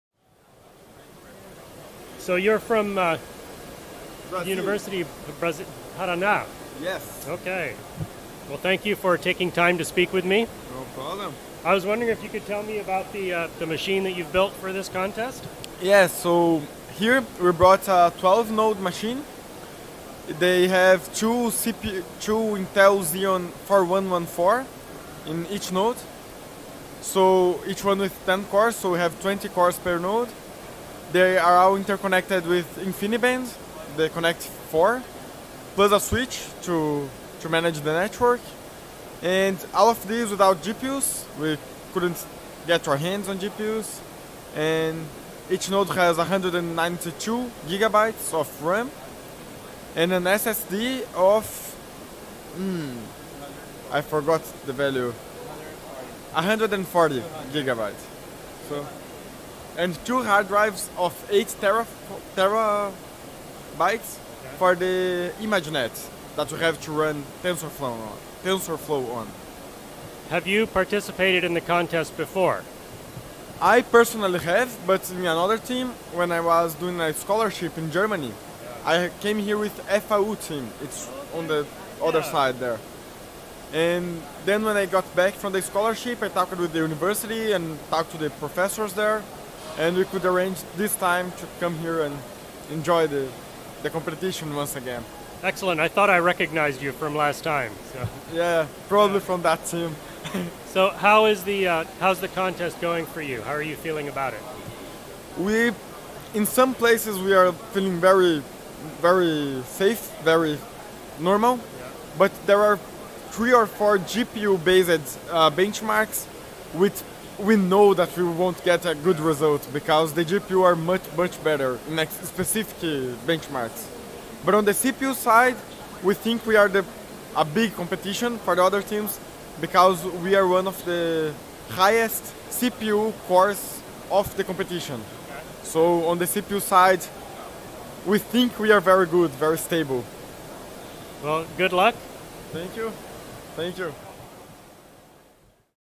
University of Parana, Brazil: Student Supercomputing
The University of Parana, Brazil, Student Supercomputing team, at isc-hpc.com ISC-HPC Frankfurt 2018